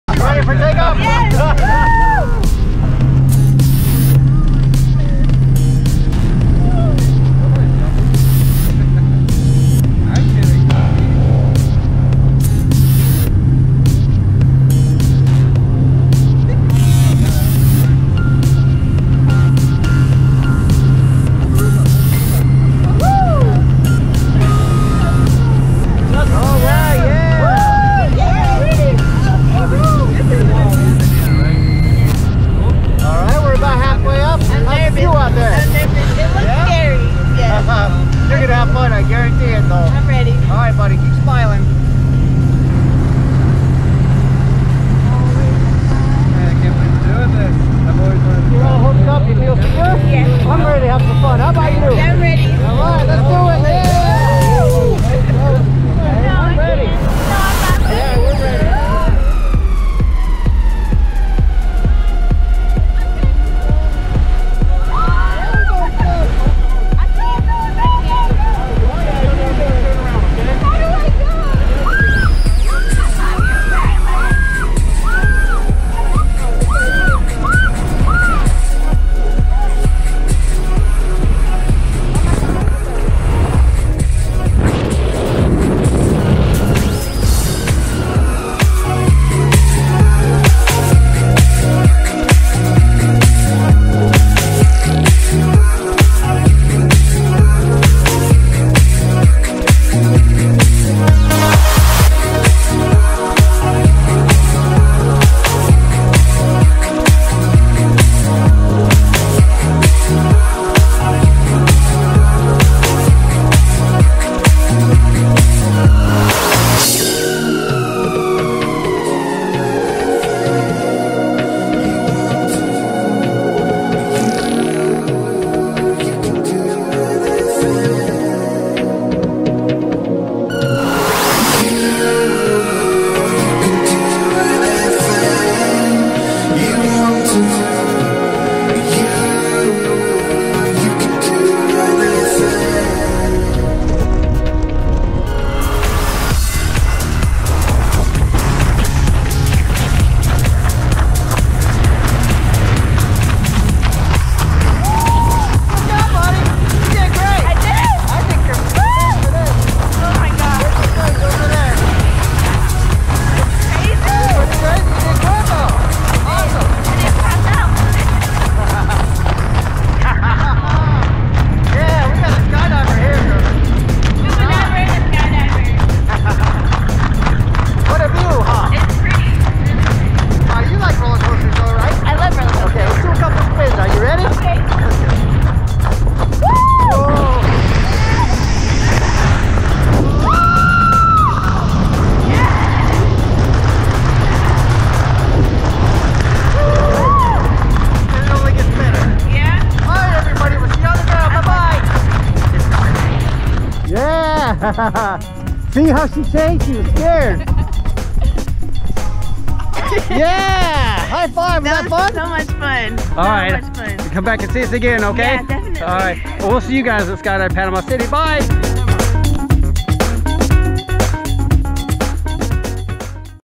My first time skydiving!
I'm adventurous, I love adrenaline rushes, I like to try new things so of course, I had to try skydiving!! I was terrified but I definitely think this is something you should try at least once in your life!!

--I couldn't breathe so that's why I looked like a fish during free fall! Don't judge me! lol